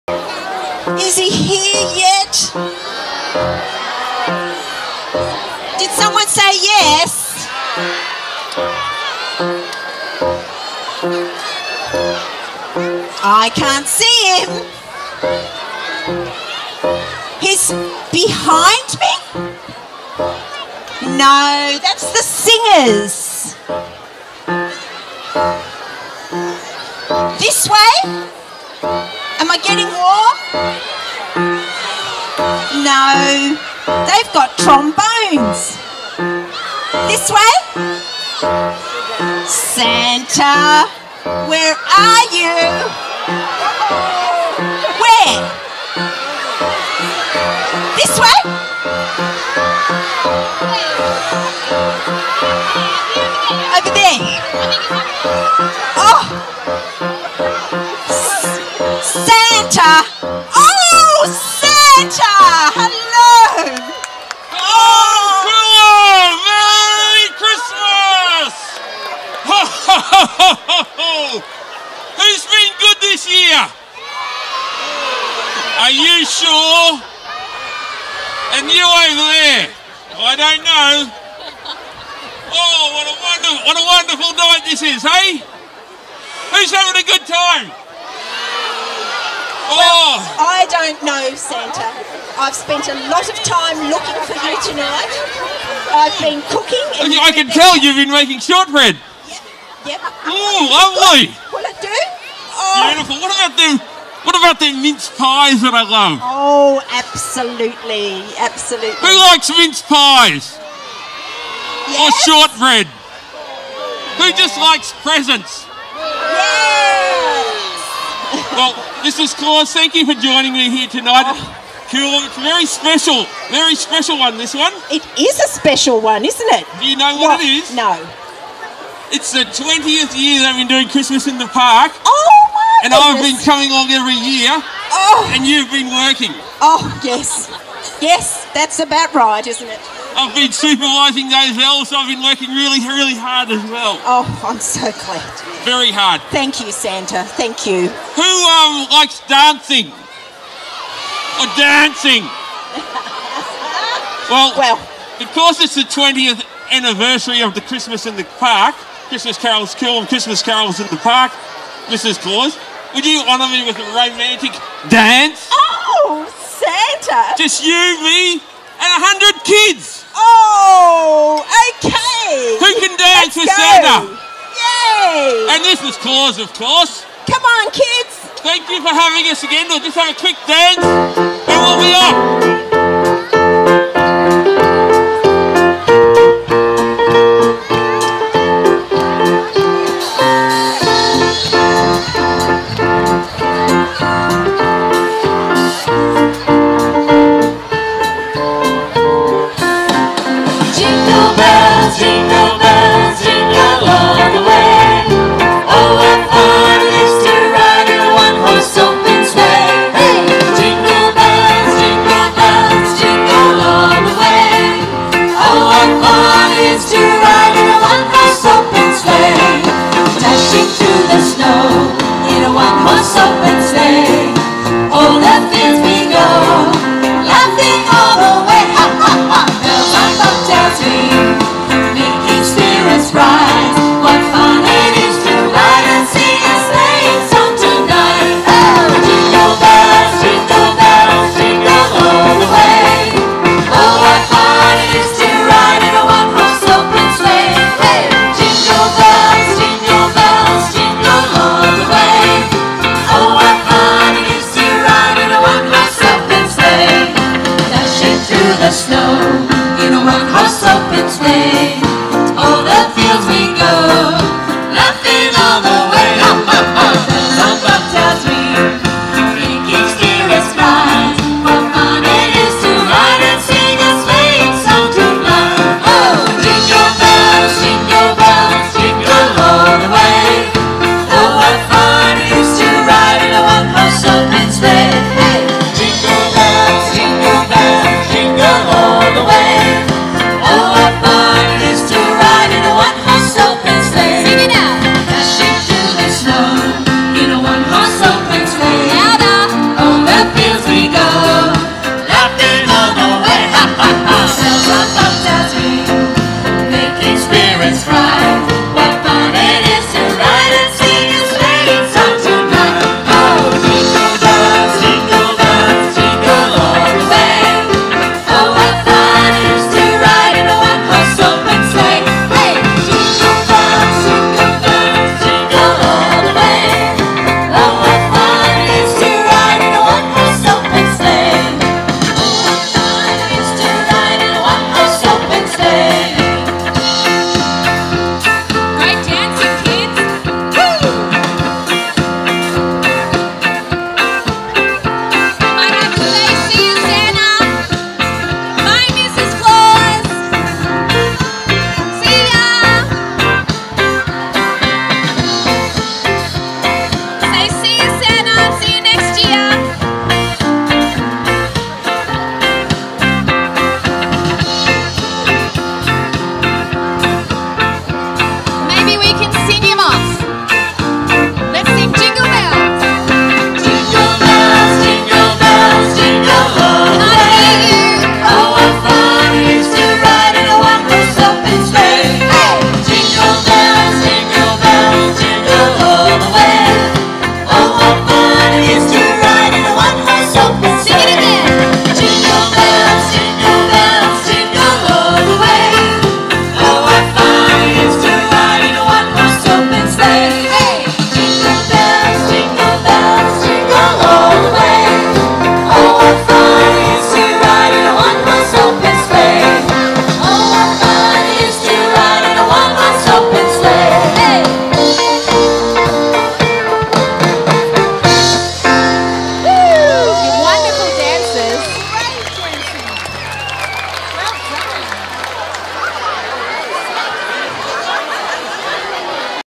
Santa Plays Hide and Seek at Coolum Beach
For Christmas in the Park 2017